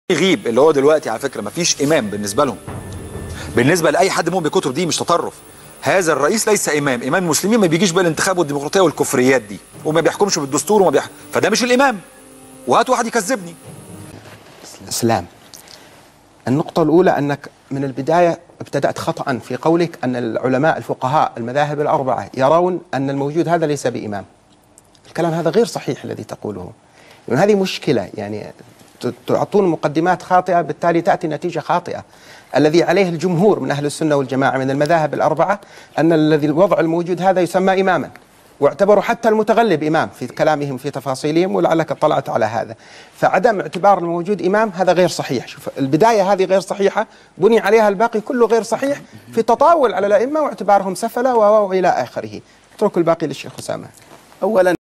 كذبة اسلام البحيري - وجود الامام عند الائمة
افتراء اسلام البحيري بخصوص / وجود الامام عند الائمة الاربعة / ورد الحبيب الجفري عليه وافحامه / وطبعا الباشا سكت زي التلميذ اللي اتمسك وهو بيغش / حشبنا الله ونعم الوكيل